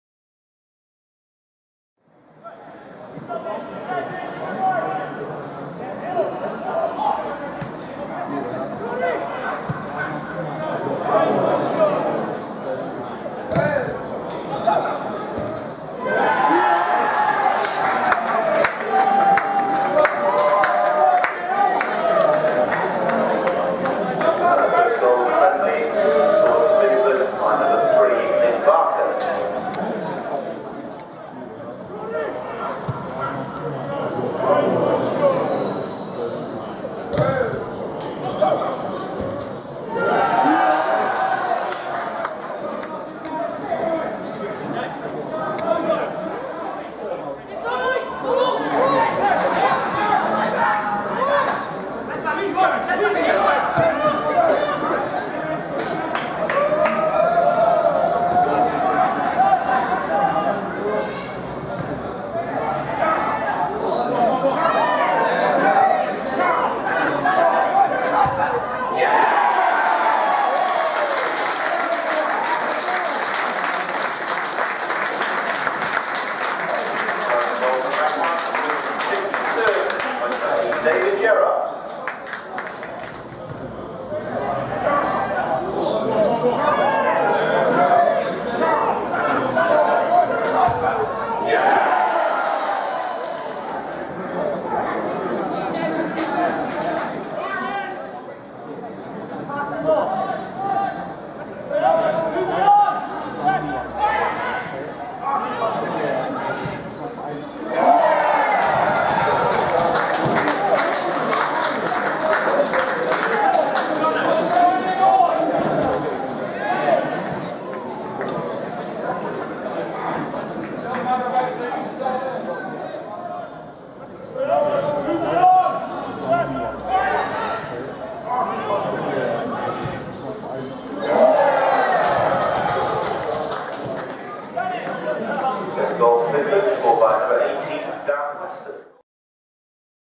Dorchester Town v Bideford AFC | 31/07/17 | Goals
Goals from the pre-season friendly between Dorchester Town and Bideford AFC on Monday 31 July 2017.